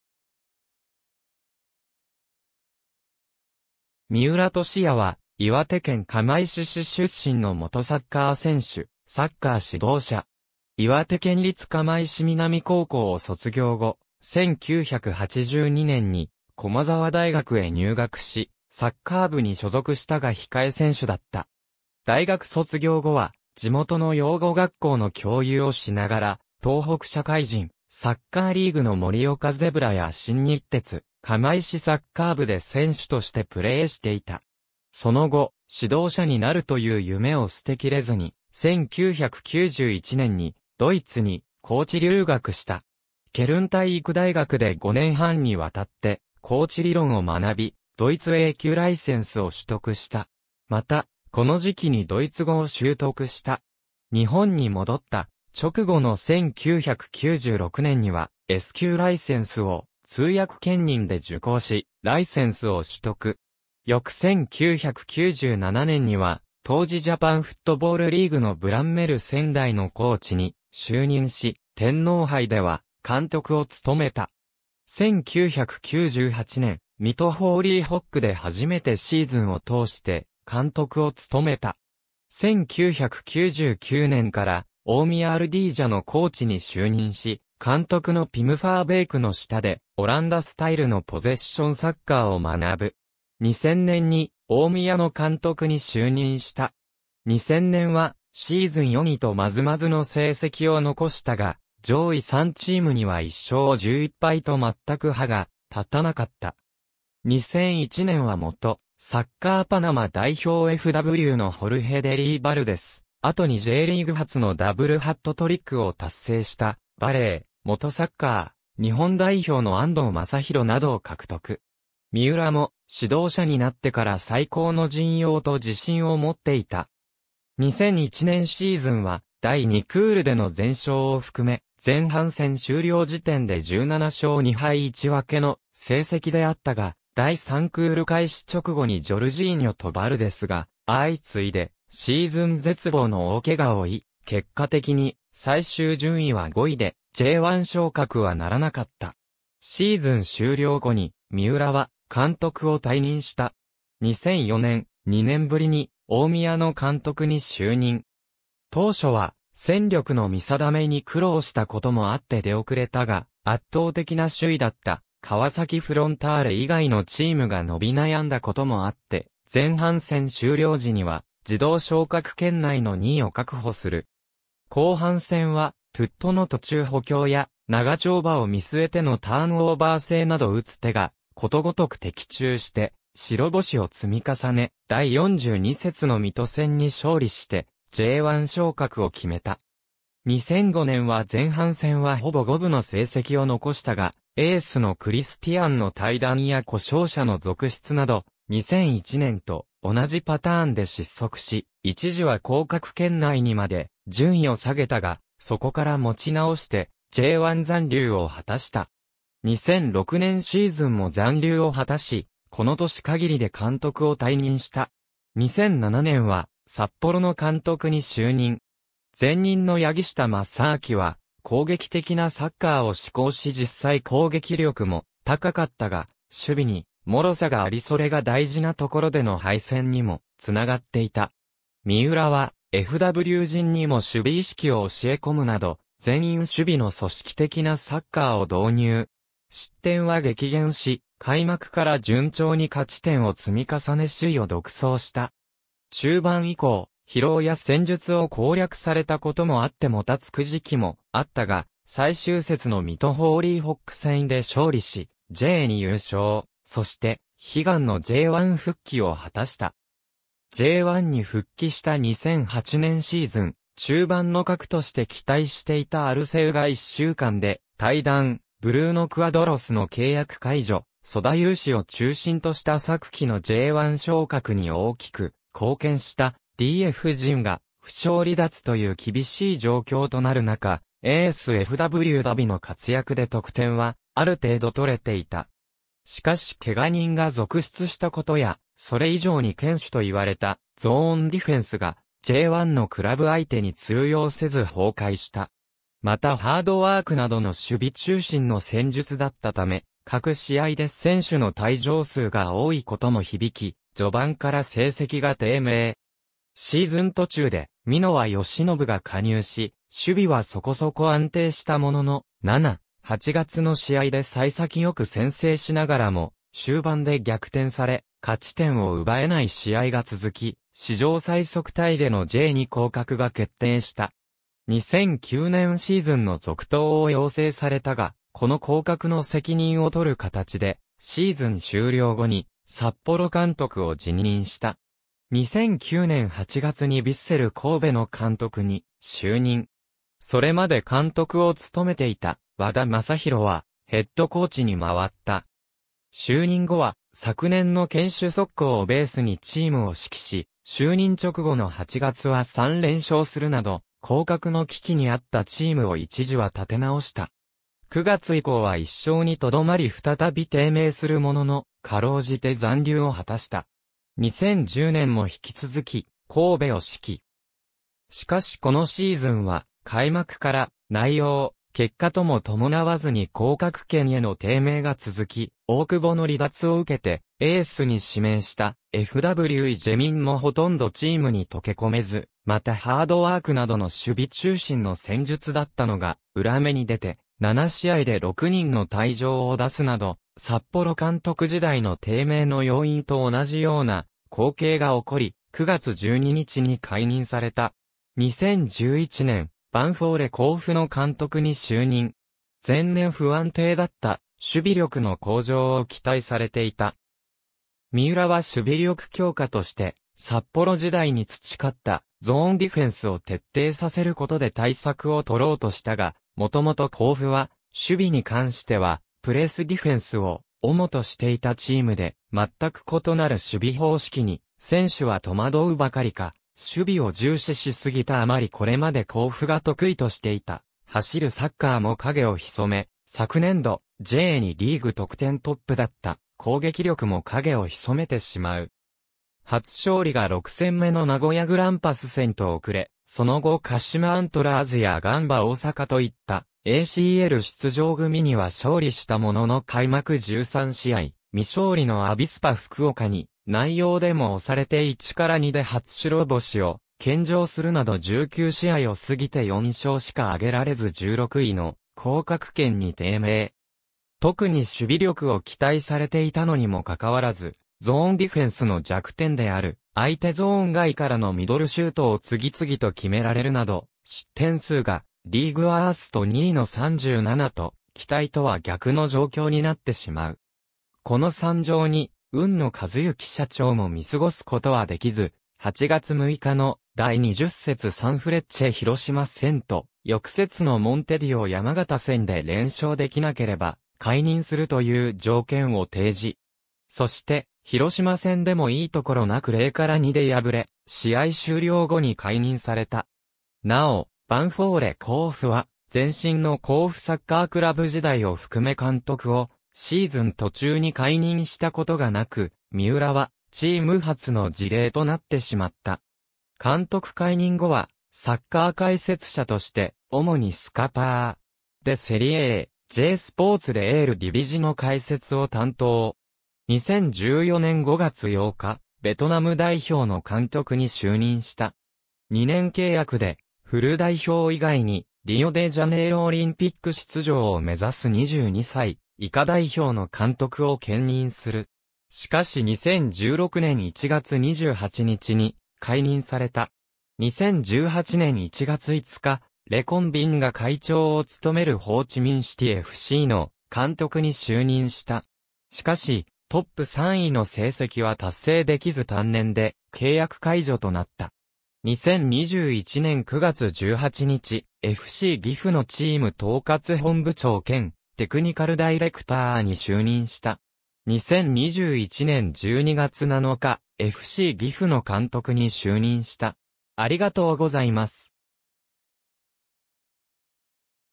三浦俊也
三浦 俊也（みうら としや、1963年7月16日-）は、岩手県釜石市出身の元サッカー選手、サッカー指導者。
岩手県立釜石南高校（現・岩手県立釜石高等学校）を卒業後、1982年に駒澤大学へ入学し、サッカー部に所属したが控え選手だった。
大学卒業後は地元の養護学校の教諭をしながら東北社会人サッカーリーグの盛岡ゼブラや新日鐵釜石サッカー部で選手としてプレーしていた。
その後、指導者になるという夢を捨てきれずに、1991年にドイツにコーチ留学した。
ケルン体育大学で5年半に渡ってコーチ理論を学び、ドイツA級（日本のA級に互等）ライセンスを取得した。
また、この時期にドイツ語を習得した。
日本に戻った直後の1996年にはS級ライセンスを通訳兼任で受講し、ライセンスを取得。
翌1997年には当時ジャパンフットボールリーグ（旧JFL）のブランメル仙台（現在のベガルタ仙台）のコーチに就任し、天皇杯では監督を務めた。
1998年、水戸ホーリーホックで初めてシーズンを通して監督を務めた。
1999年から大宮アルディージャのコーチに就任し、監督のピム・ファーベークの下でオランダスタイルのポゼッションサッカーを学ぶ。
2000年に大宮の監督に就任した。
2000年はシーズン4位とまずまずの成績を残したが、上位3チームには1勝11敗と全く歯が立たなかった。
2001年は元サッカーパナマ代表FWのホルヘ・デリー・バルデス、後にJリーグ初のダブルハットトリックを達成したバレー、元サッカー日本代表の安藤正裕などを獲得。
三浦も「指導者になってから最高の陣容」と自信を持っていた。
2001年シーズンは第2クールでの全勝を含め、前半戦終了時点で17勝（延長勝ち4）2敗1分けの成績であったが、第3クール開始直後にジョルジーニョとバルデスが相次いでシーズン絶望の大ケガを負い、結果的に最終順位は5位でJ1昇格は成らなかった。
シーズン終了後に三浦は監督を退任した。
2004年、2年ぶりに大宮の監督に就任。
当初は戦力の見定めに苦労したこともあって出遅れたが、圧倒的な首位だった川崎フロンターレ以外のチームが伸び悩んだこともあって前半戦終了時には自動昇格圏内の2位を確保する。
後半戦はトゥットの途中補強や、長丁場を見据えてのターンオーバー制など打つ手がことごとく的中して白星を積み重ね、第42節の水戸戦に勝利してJ1昇格を決めた。
2005年は前半戦はほぼ五分の成績を残したが、エースのクリスティアンの退団や故障者の続出など、2001年と同じパターンで失速し、一時は降格圏内にまで順位を下げたが、そこから持ち直してJ1残留を果たした。
2006年シーズンも残留を果たし、この年限りで監督を退任した。
2007年は札幌の監督に就任。
前任の柳下正明は攻撃的なサッカーを志向し実際攻撃力も高かったが、守備に脆さがありそれが大事なところでの敗戦にもつながっていた。
三浦はFW陣にも守備意識を教え込むなど全員守備の組織的なサッカーを導入。
失点は激減し開幕から順調に勝ち点を積み重ね首位を独走した。
中盤以降、疲労や戦術を攻略されたこともあってもたつく時期もあったが、最終節（12月1日）の水戸ホーリーホック戦で勝利し、J2優勝、そして悲願のJ1復帰を果たした。
J1に復帰した2008年シーズン、中盤の核として期待していたアルセウが1週間で退団、ブルーノ・クアドロスの契約解除、曽田雄志を中心とした昨季のJ1昇格に大きく貢献したDF陣が負傷離脱という厳しい状況となる中、エースFWダヴィの活躍で得点はある程度取れていた。
しかしケガ人が続出したことや、それ以上に堅守と言われたゾーンディフェンスがJ1のクラブ相手に通用せず崩壊した。
またハードワークなどの守備中心の戦術だったため、各試合で（1試合で二人以上出た試合もあった）選手の退場数が多いことも響き、序盤から成績が低迷。
シーズン途中で箕輪義信が加入し、守備はそこそこ安定したものの、7、8月の試合で幸先良く先制しながらも終盤で逆転され、勝ち点を奪えない試合が続き、史上最速タイ（当時・残り5試合）でのJ2降格が決定した。
2009年シーズンの続投を要請されたが、この降格の責任を取る形でシーズン終了後に札幌監督を辞任した。
200...